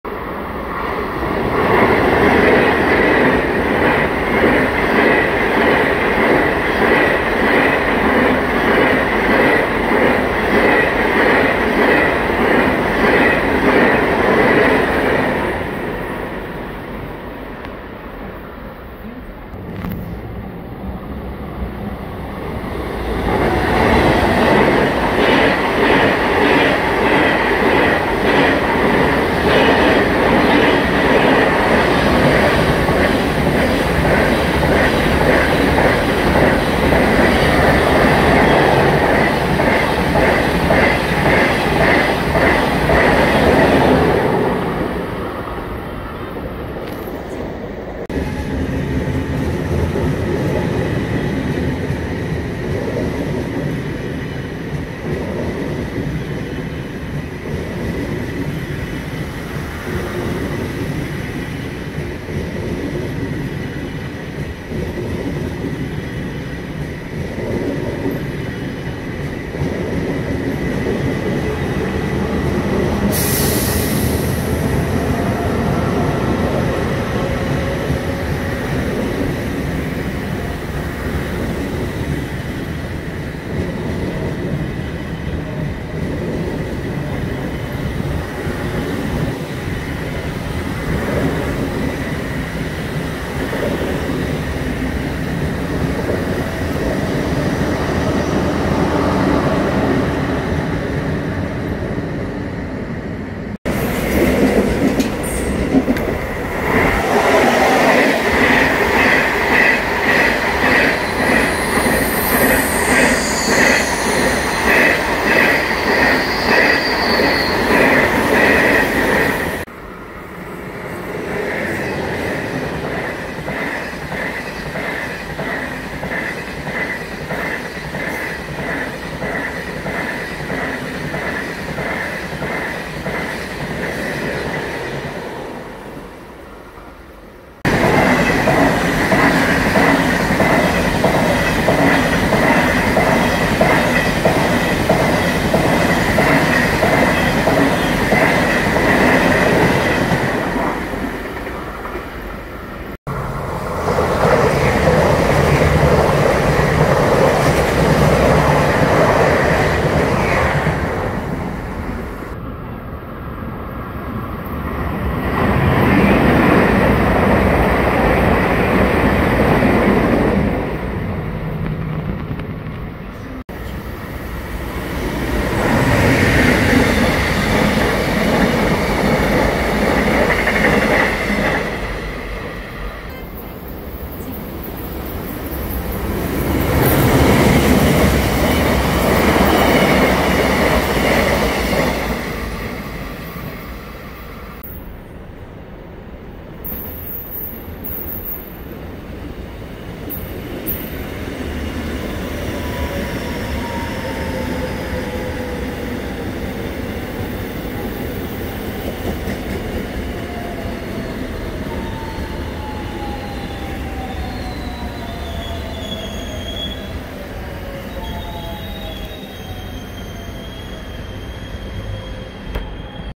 KTX.Saemaul, Mugunghwa. Train passing through Gasan Digital Station on Subway Line 1
00:00  star
00:45  KTX driving video
02:32  Driving Video of Saemaul Train
02:53  Mugunghwa Train Video

These are the driving images of trains taken at the station of Subway Line 1, where all trains leaving Seoul pass through. The Saemaeul train was the highest-class train before the KTX train, followed by Mugunghwa train.